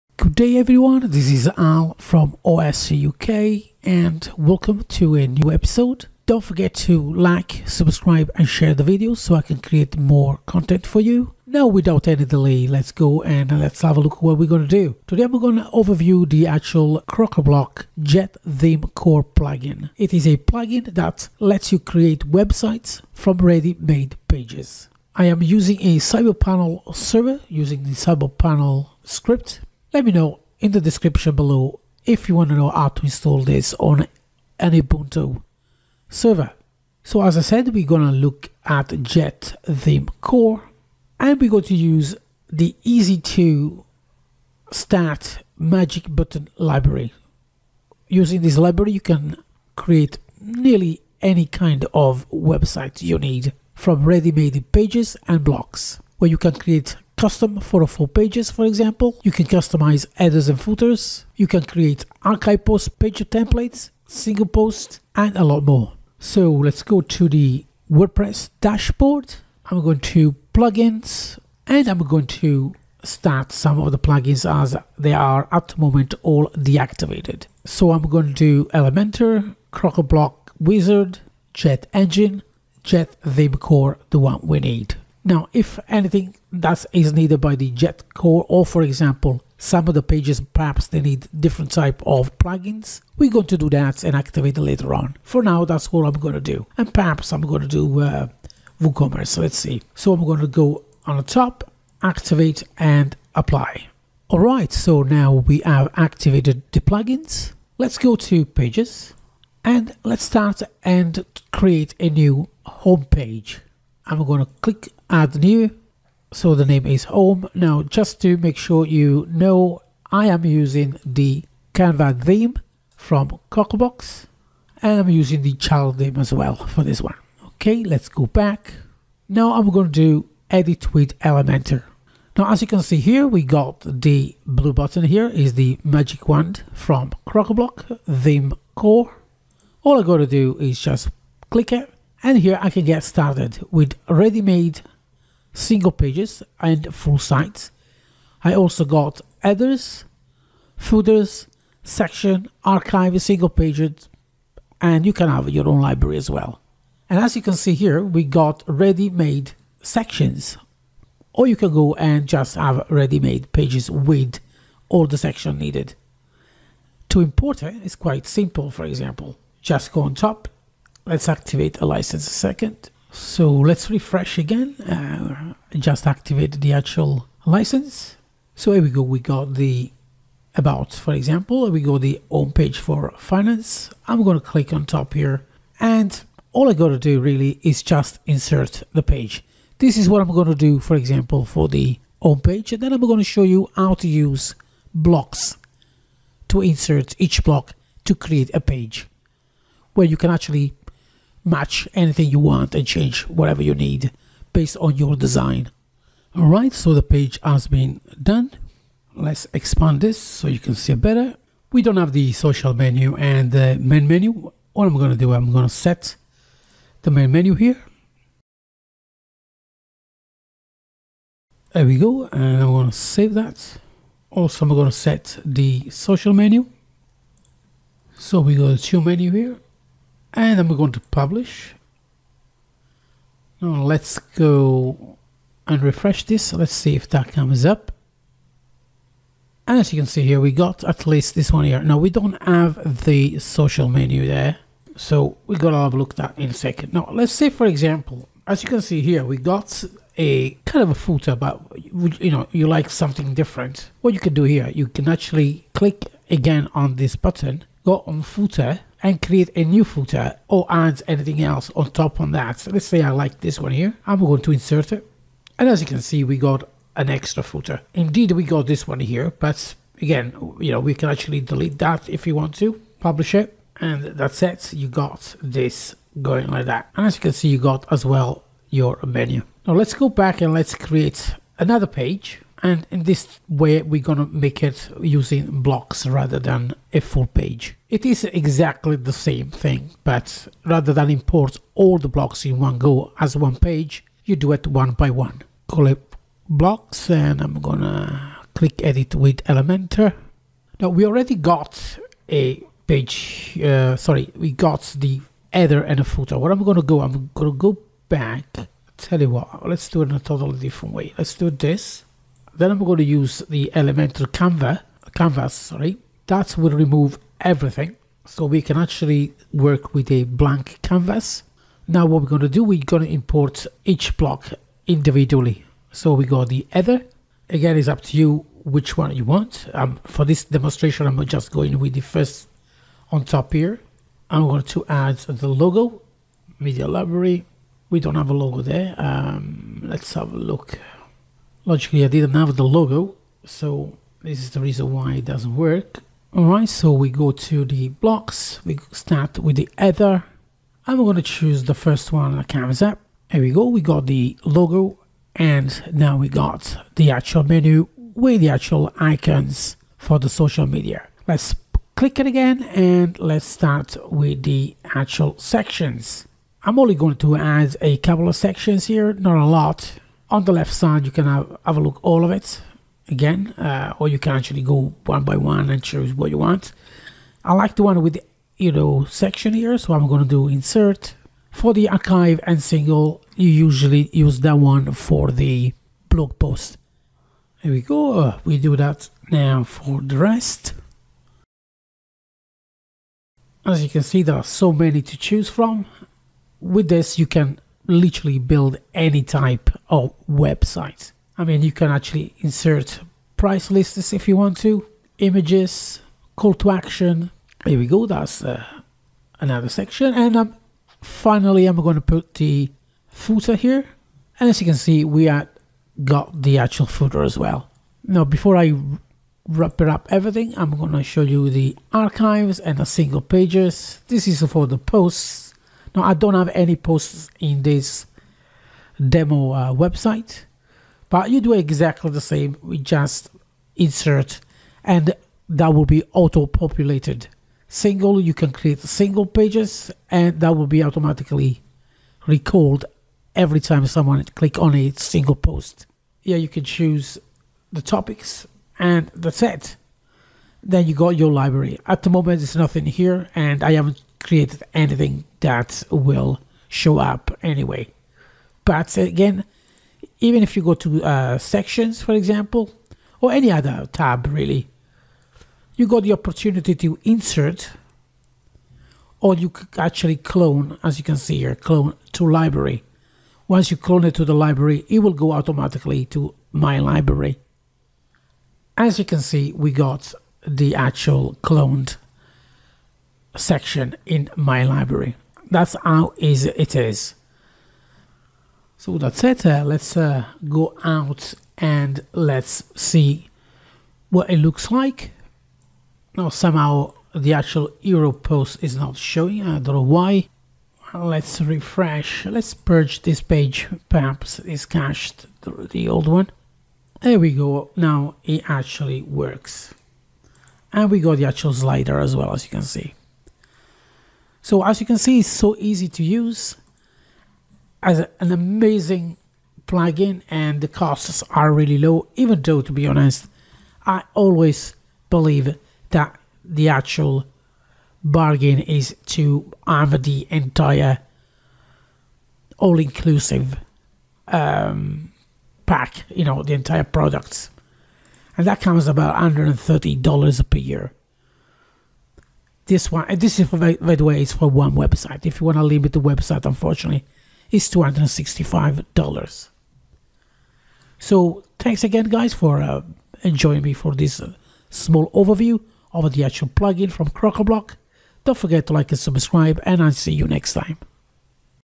JetThemeCore plugin for Elementor | Magic Button Library overview
Did you know that you can design your entire website using JetThemeCore for Elementor and its Magic Button Library?

Watch the video and learn more about one of the coolest Crocoblock plugin.

By the way, JetThemeCore is 100% FREE! You can get it with Crocoblock subscription

PLEASE HELP US TO UNDERSTAND MORE ON HOW TO MAKE THIS CHANNEL BETTER
HERE IS A QUICK SURVEY. THANK YOU FOR HELPING